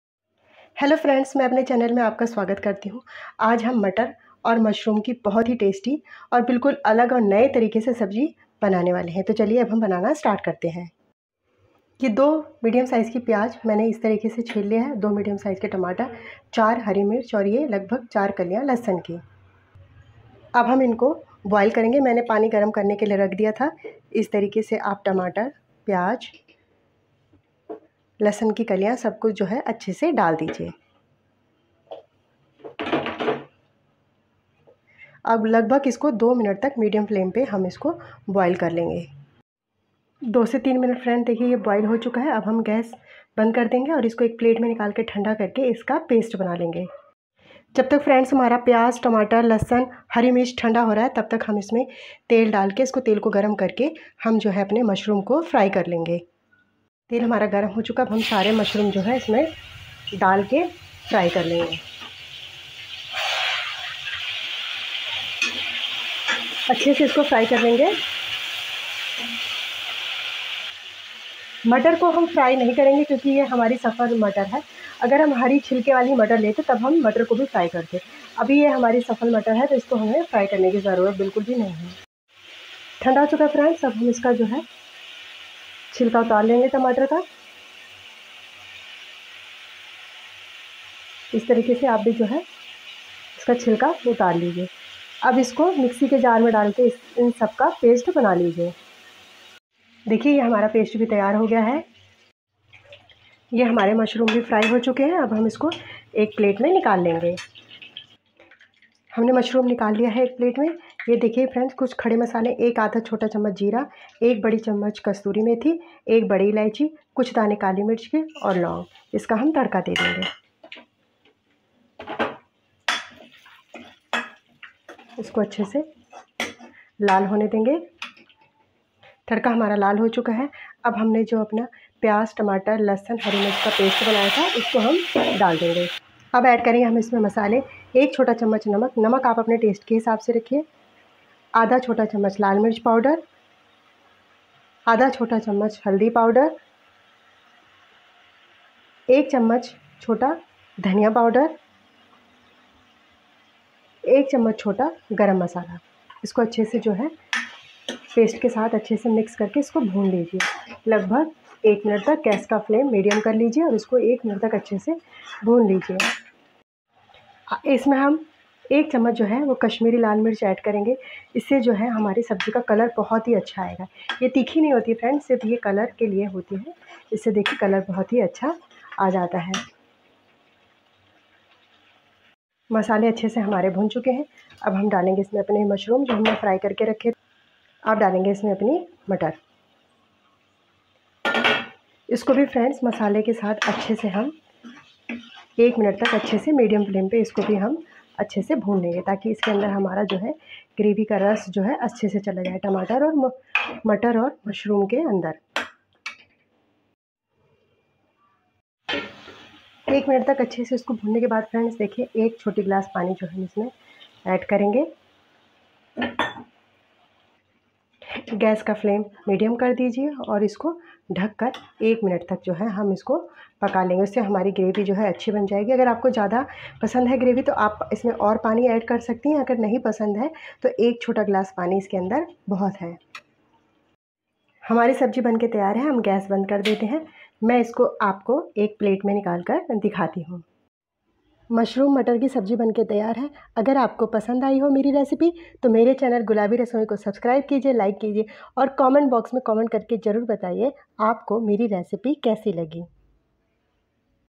Mushroom मटर स्वादिष्ट सब्जी banaaiye 5 मिनट में😋 how to make matar mushroom recipe
Mushroom मटर स्वादिष्ट सब्जी banaaiye सिर्फ 5 मिनट में😋 restaurant style Mushroom Matar

Maine Apni video mein restaurant style Mushroom Matar ki sabji banana Sikhaya hai mujhe ummid hai aapko Meri recipe Pasand Aaegi

Mushroom Matar ki sabji Kaise

how to make recipe in Hindi Mushroom Matar

Gulabi'scorner